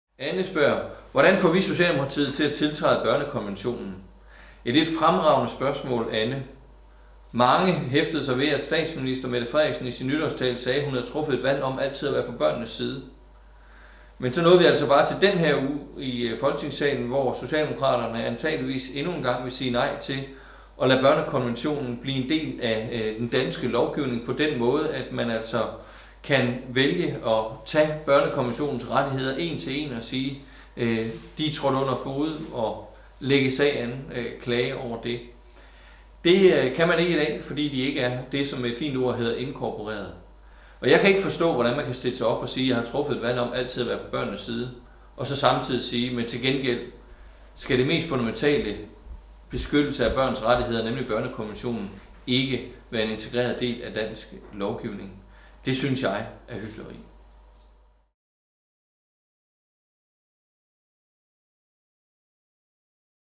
Spørg Morten Østergaard : Bør Danmark skrive børnekonventionen ind i dansk lov?
Videoen stammer fra en live-udsendelsen, som Morten Østergaard havde på Facebook den 16. januar 2020.